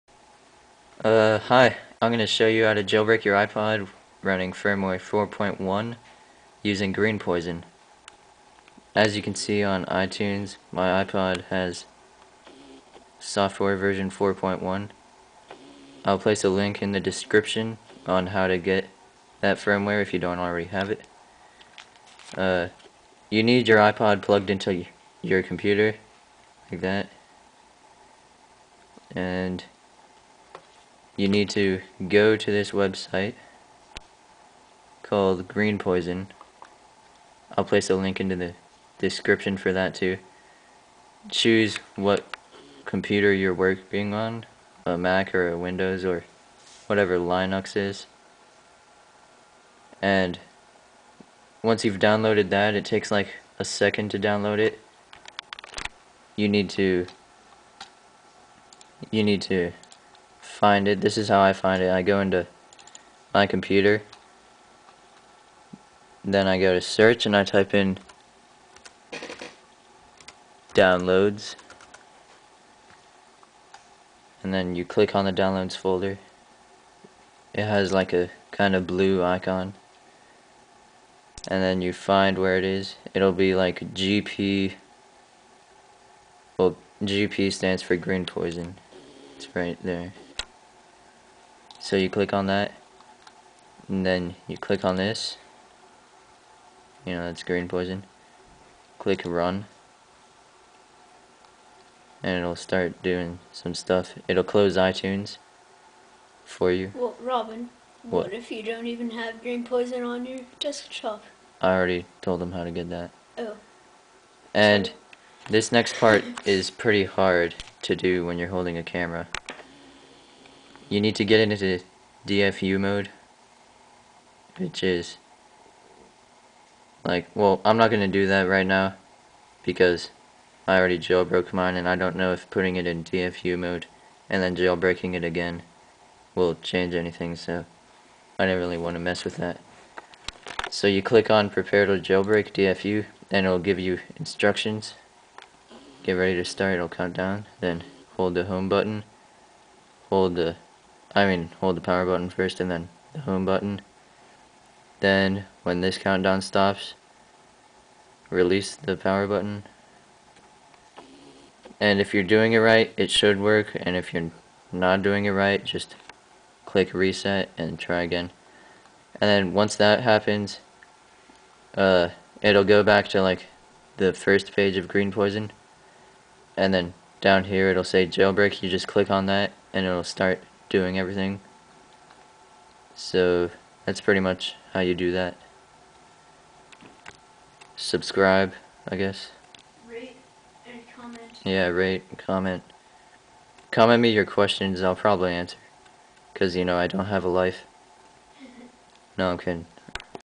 how to jailbreak using greenpoison with firmware 4.1 works the FIRST TIME!
This is how I jailbroke my ipod touch 2nd gen. I forgot to mention in the video that before you click"prepare to jailbreak" you need to turn your device off. after dfu mode click on "jailbreak"

(if you were wondering my theme is called "basketball")